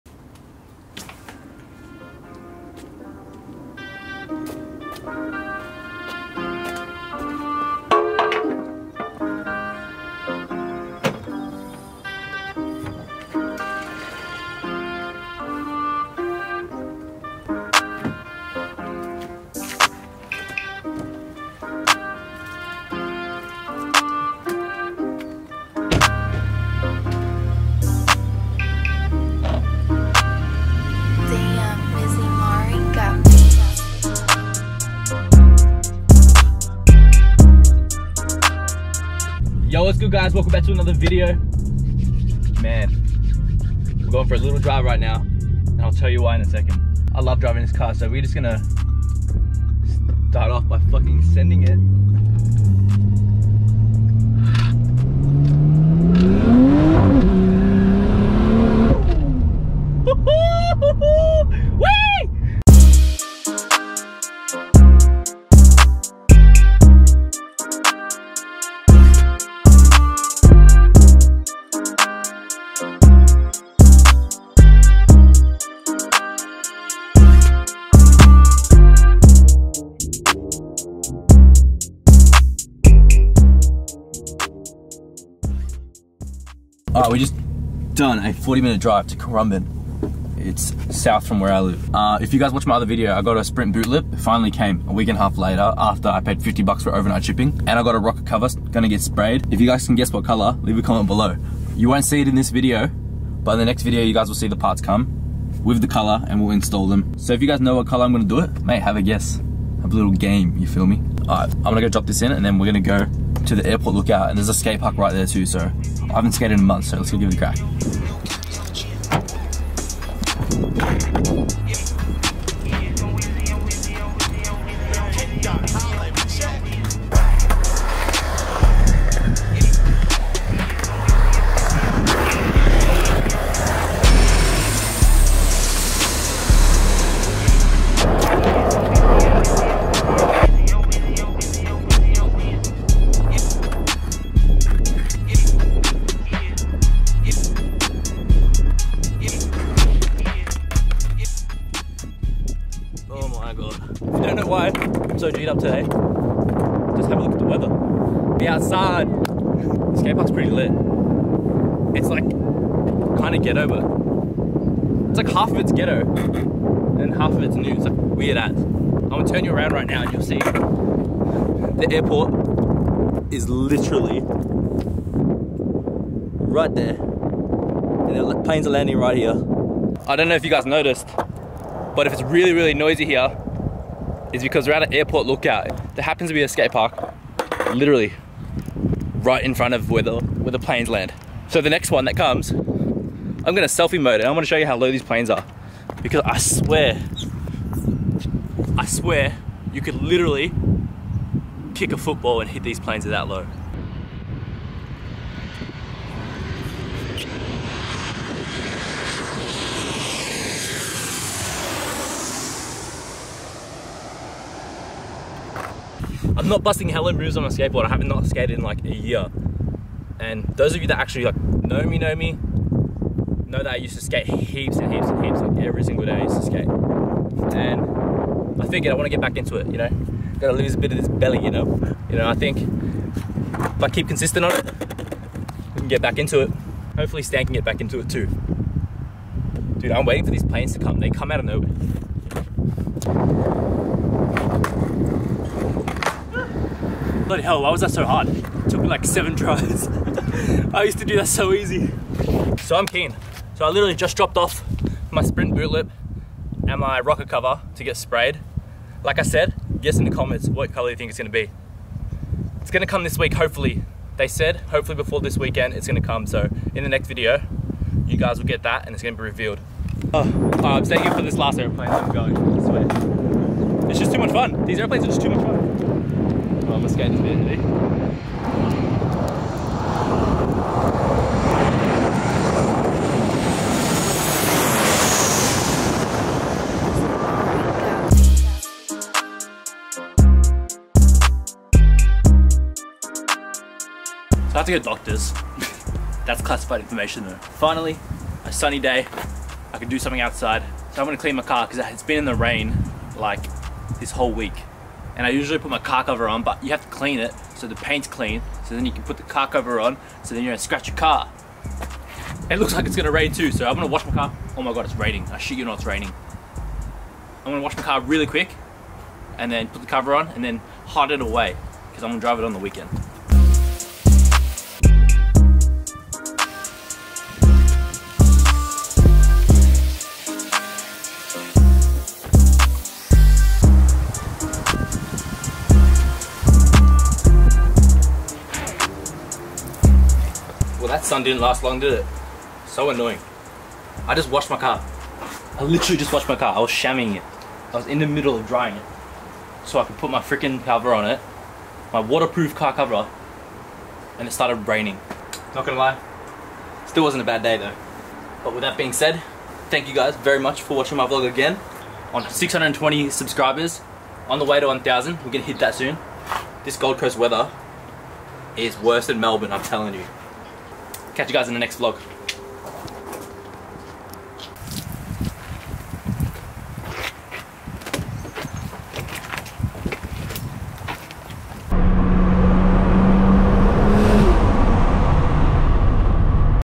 THE BEST AIRPORT LOOKOUT (vlog)
Hey guys!
Dropped a few car parts off to get sprayed and had a good sunny day.

(let me know what videos you want to see)

gyonbrouge & gyonslens all socials